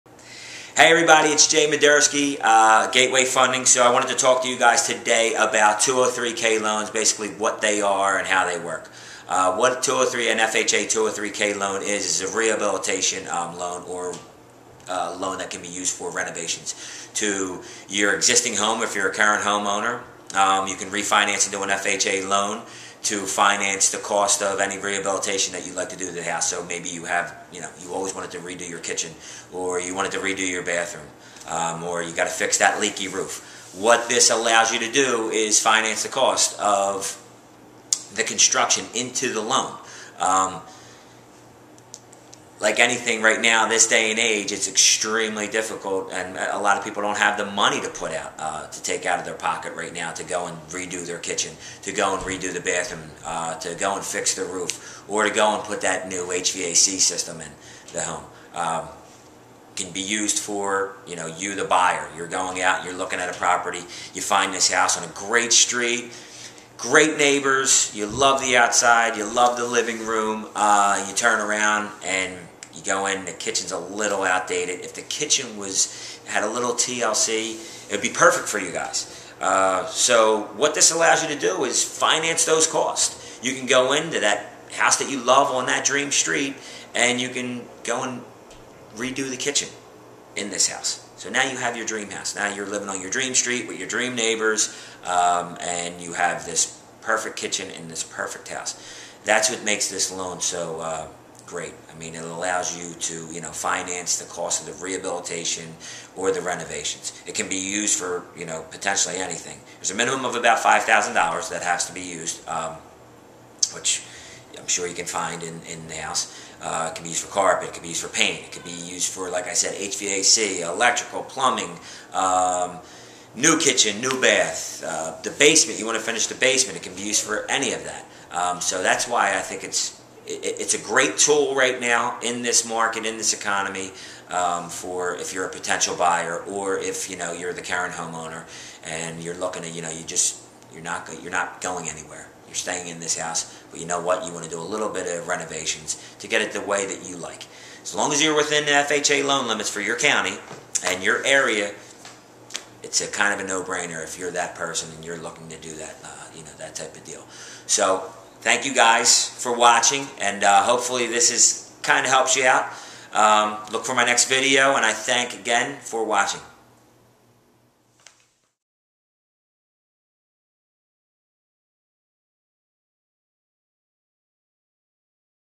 Jay Moderski - FHA203K Loan
- How many times have you paraded through a house while looking for the perfect one and come across the ALMOST perfect one?  Would that almost perfect home be THE ONE if it had a nicer kitchen?  A larger bedroom or even a finished basement?  Consider this: You can build the renovation and improvements right into the mortgage -- and never have to worry about out-of-pocket expenses up front.  There are other advantages to the 203(k) loan too.

Jay Moderski
Branch Manager and
Mortgage Originator
NMLS #: 133515